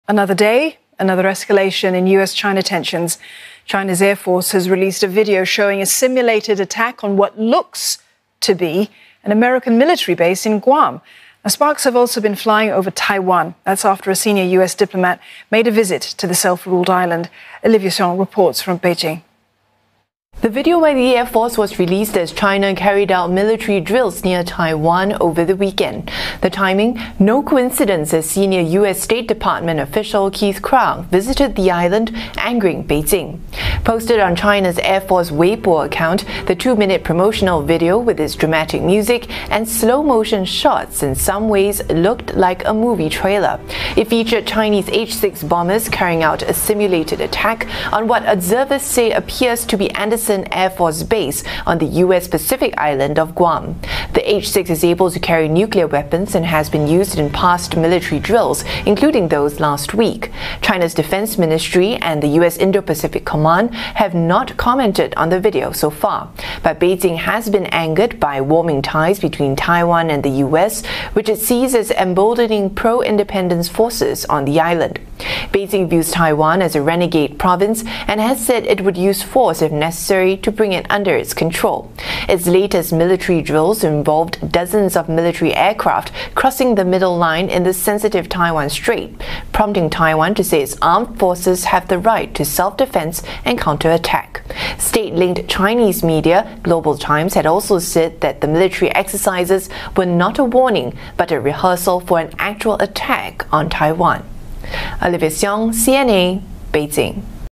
China’s air force video appears to show simulated attack on US base in Guam
China's air force has released a video showing a simulated attack on what looks to be an American military base in Guam.